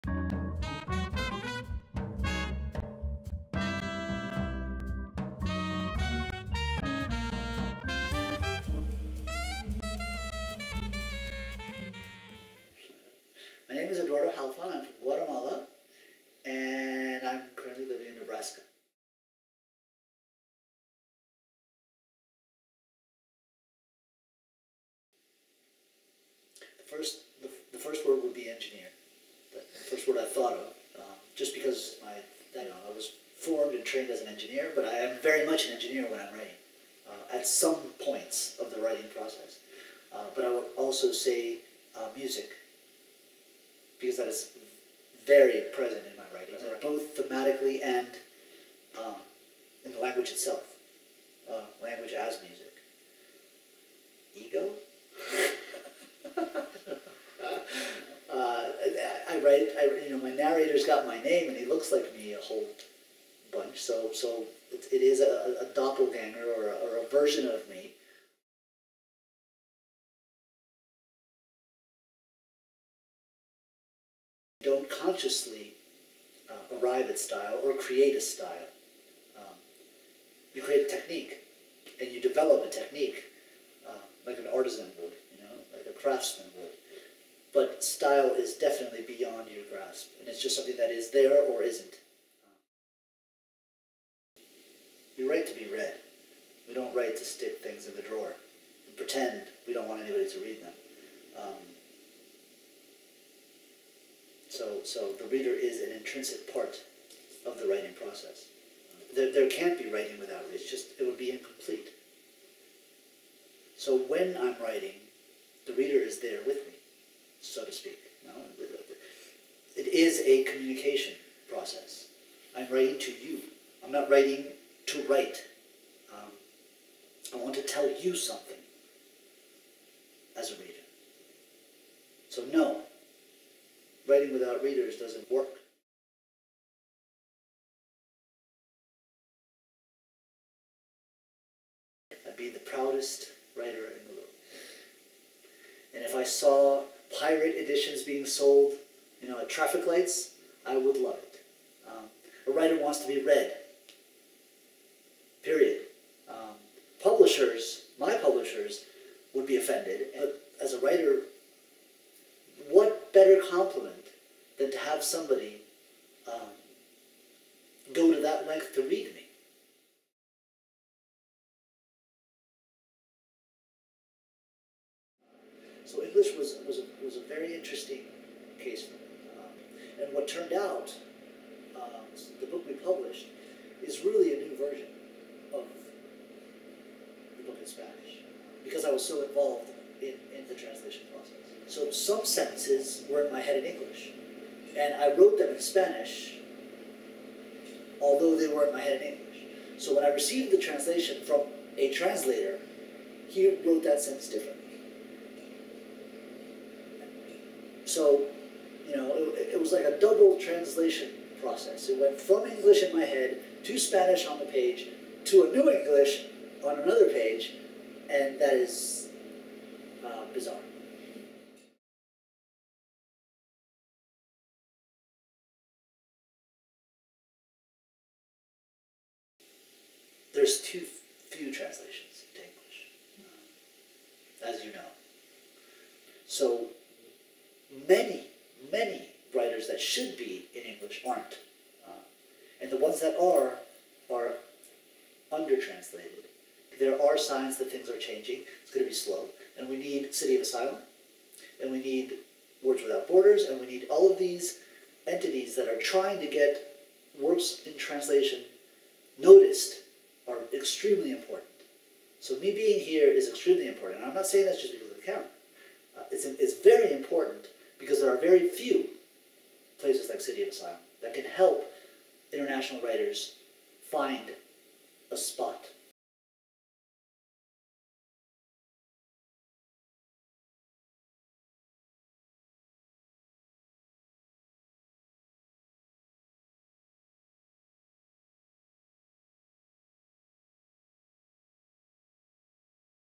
The Writer's Block: A Video Q&A with Eduardo Halfon
Eduardo Halfon is the author of The Polish Boxer, which was recently published in English by Bellevue Literary Press. On February 12, City of Asylum/Pittsburgh hosted a reading and Q&A for Halfon. Prior to this, Halfon was able to sit down with Sampsonia Way and talk about the craft of writing.